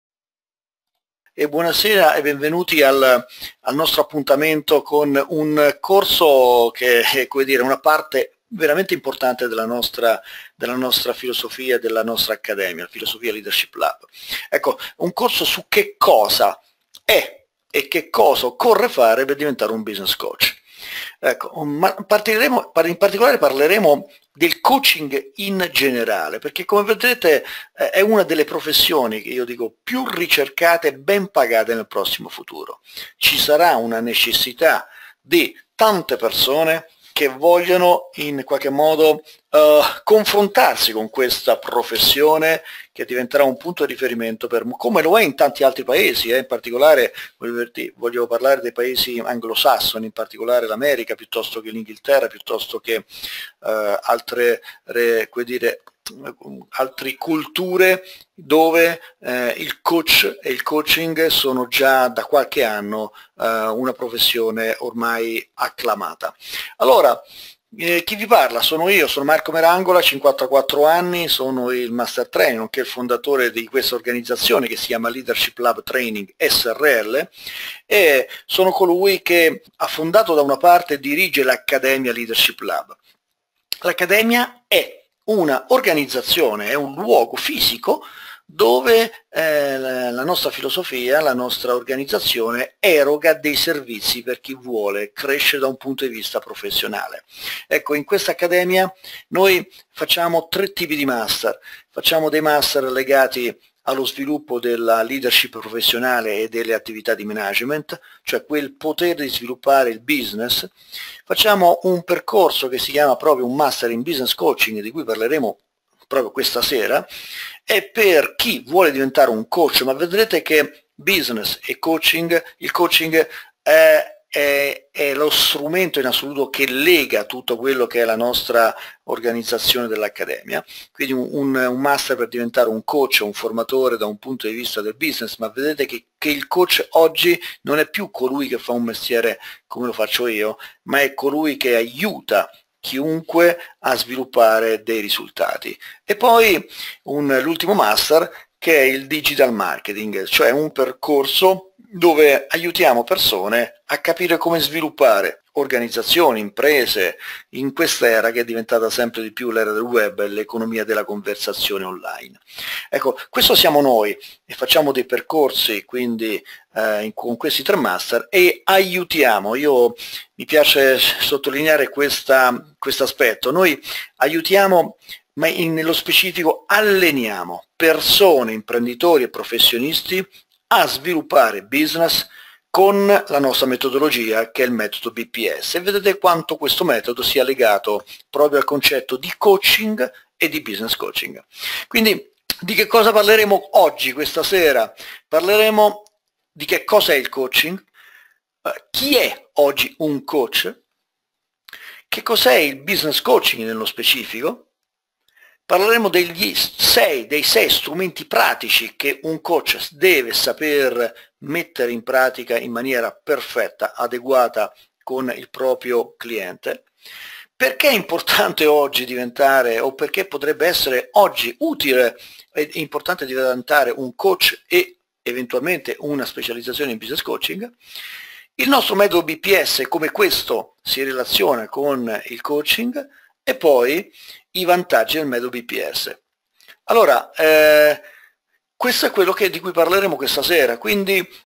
COME DIVENTARE UN BUSINESS COACH - INTRO - Corso Online
INTRODUZIONE del Corso Online "DIVENTA BUSINESS COACH" svolto da Marco Merangola.

Puoi continuare a vedere il Corso completo presso l'area delle Risorse formative dedicate alla tua crescita Professionale cliccando qui:
Grazie e ci vediamo al prossimo corso!

Team LLAB